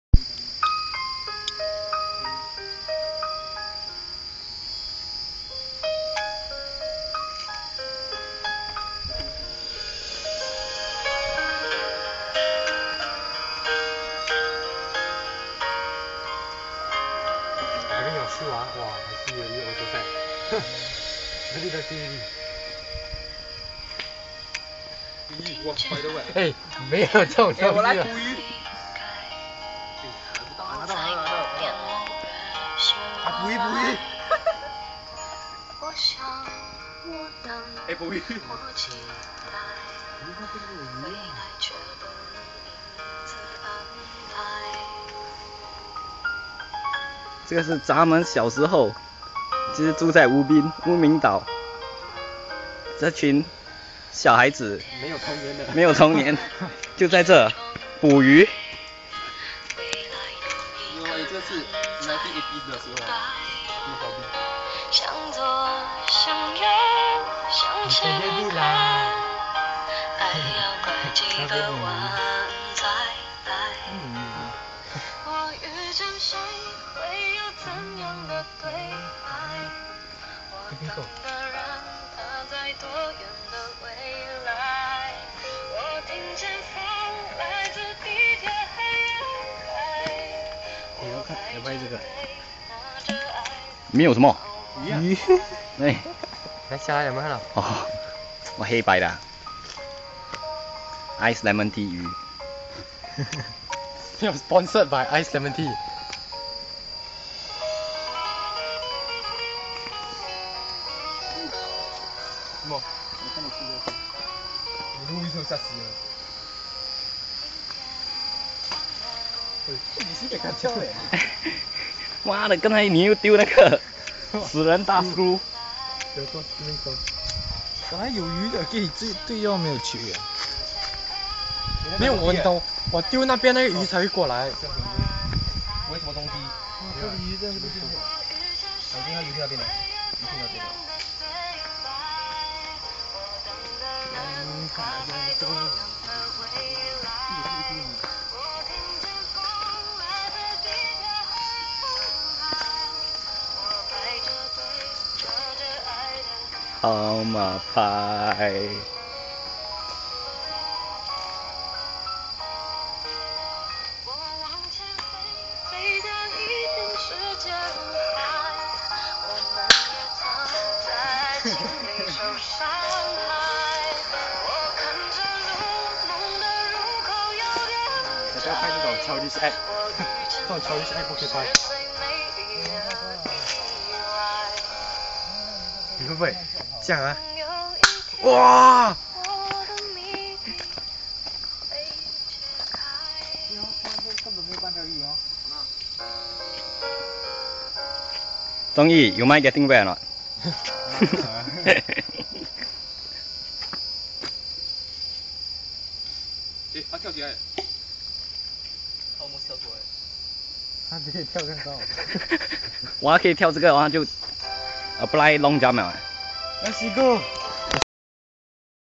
Ubin fishing trip MTV
a group of boys saw a lake, and began fishing.. they came together and do up a MTV..